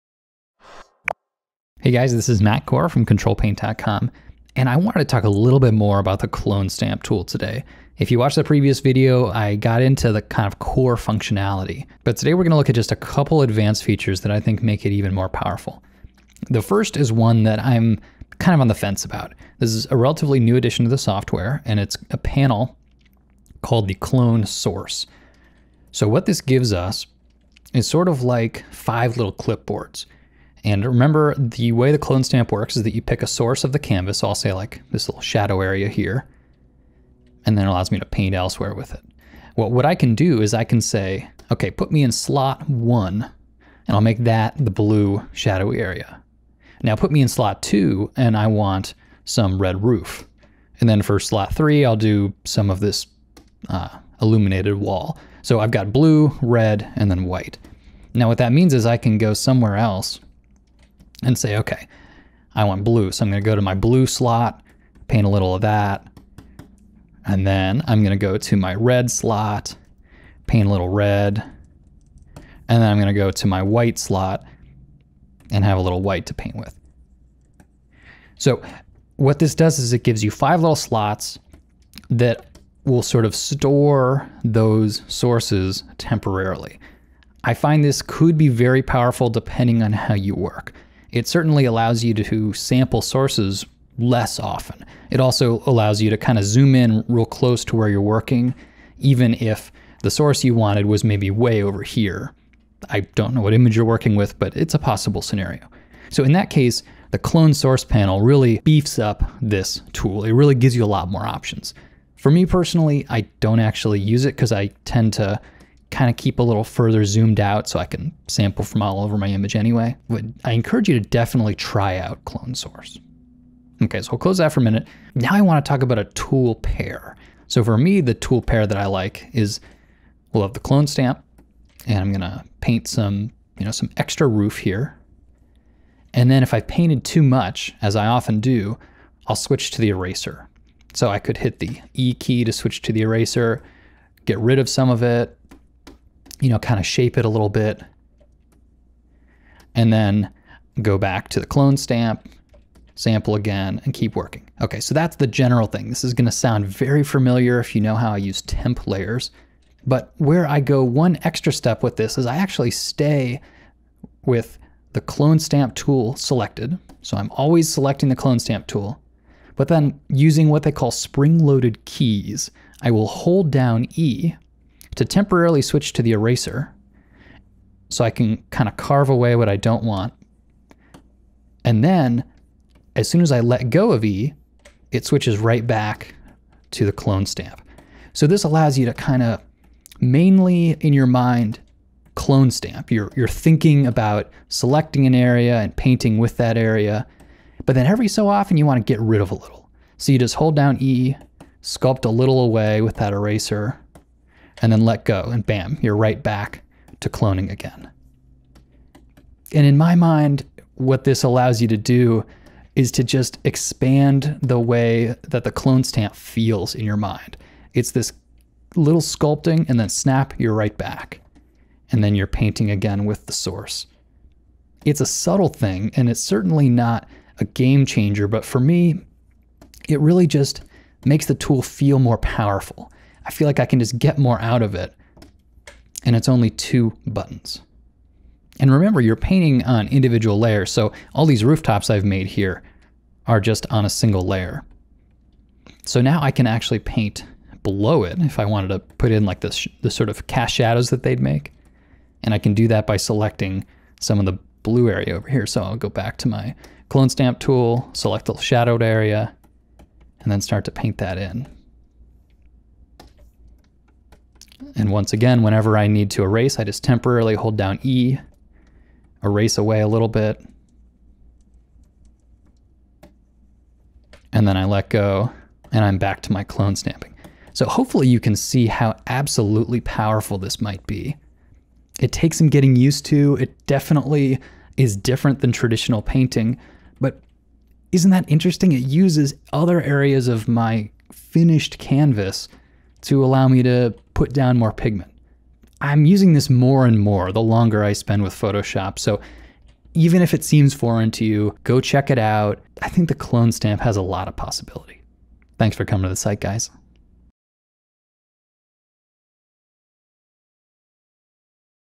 Painting with the Clone Stamp pt.2
In the previous video I introduced the clone stamp as a useful tool for digital painters.  In this demonstration I explore some of the more advanced features, and show how powerful it can be for illustration.   If you have any additional tips, we'd love to hear them in the comments!